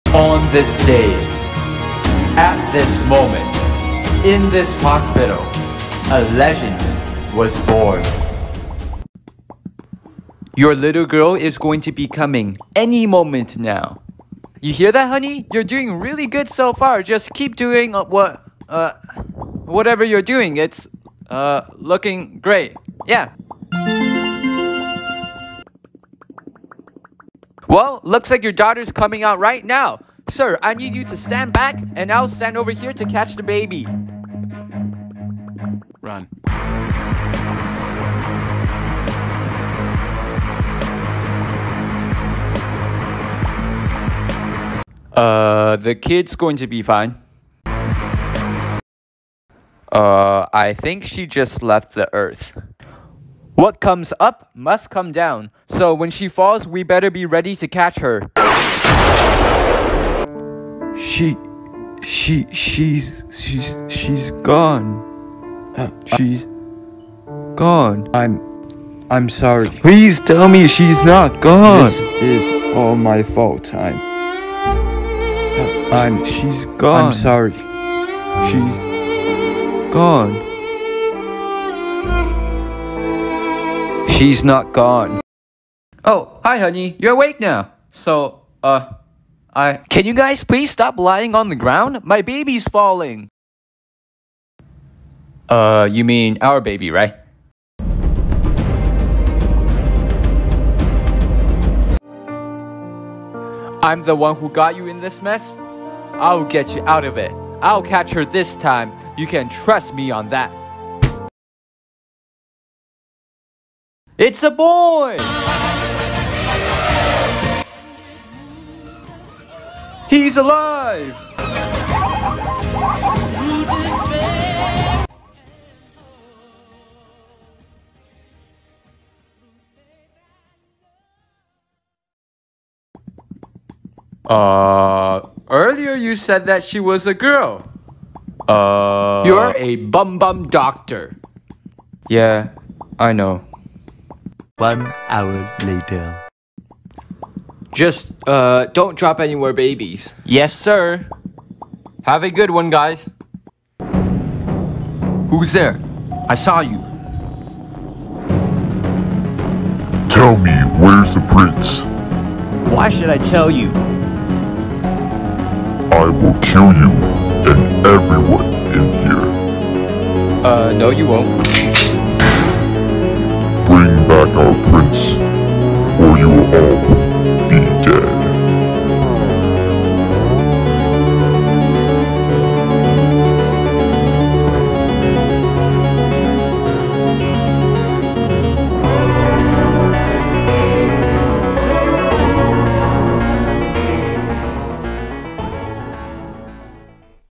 Rise Of PopCat Ep1: Birth Of A Legend
First episode of the series (Rise Of PopCat). This is a series I've started, and it tells of the Rise Of PopCat. This takes place in a medival-like modern world. Genre is Fantasy, Comedy, War, Adventure, and Memes :p if memes even is a genre. If you guys enjoyed it, I will be making more episodes, while also posting memes in between schedule :D

IF I FORGOT ANYONE PLZ LET ME KNOW IN COMMENTS :3